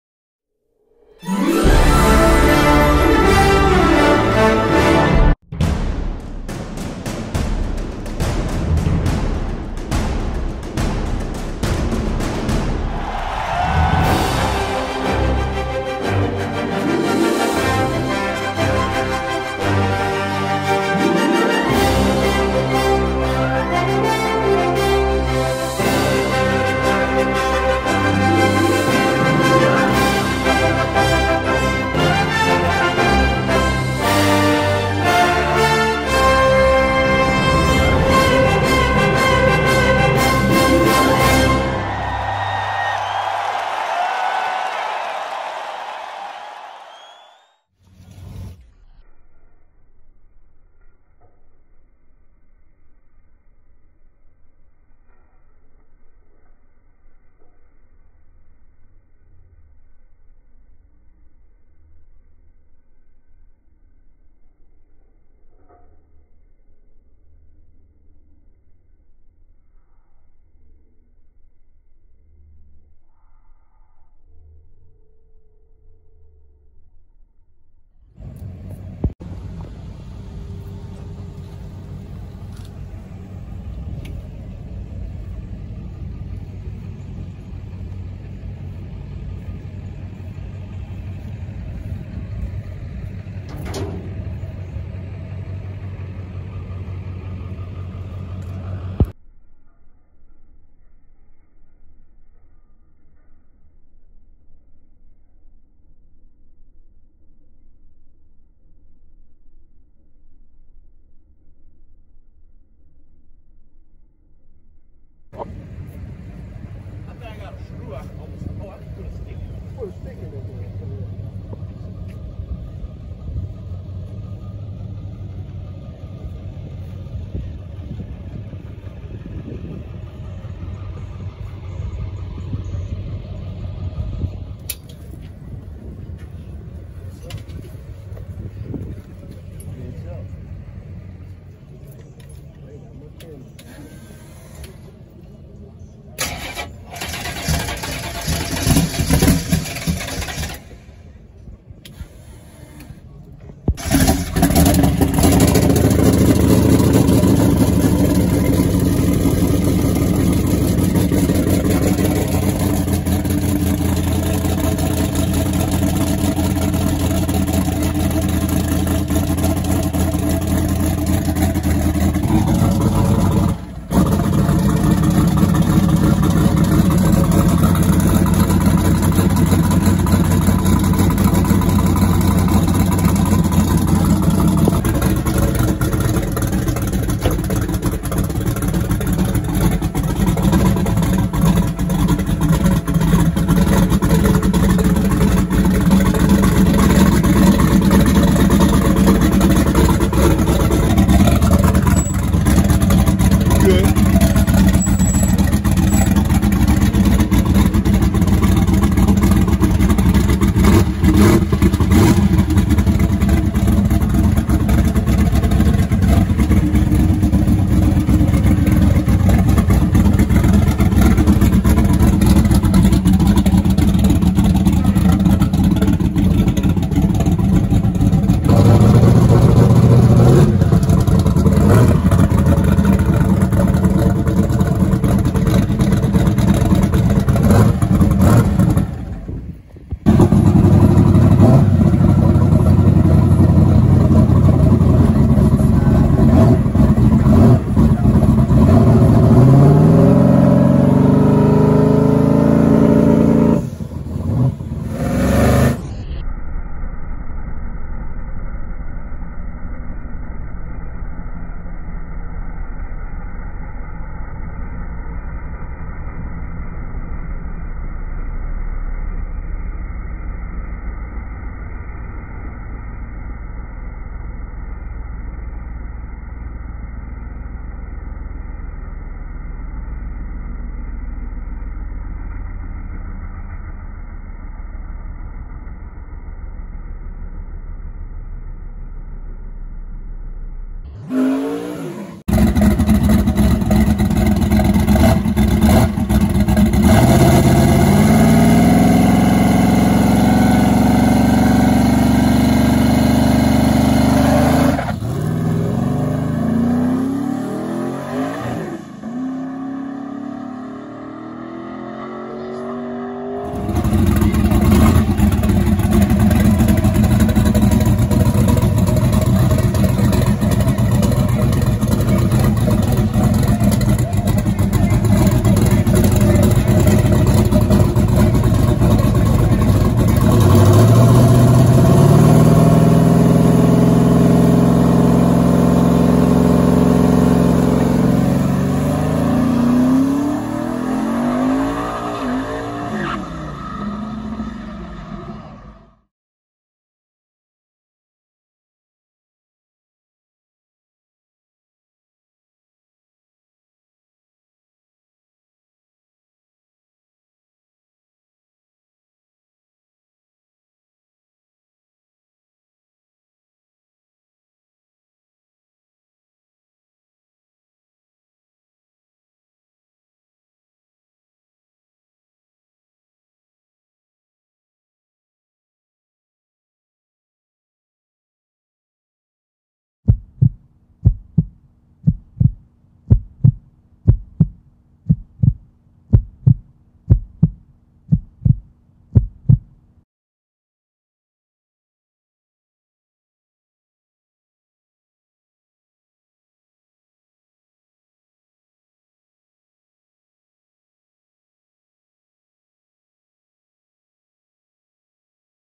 "Iron Man" Malibu At Street Beast 5
New Turbo Malibu On Rims Getting Down At SB5
Driver IG : @levencio_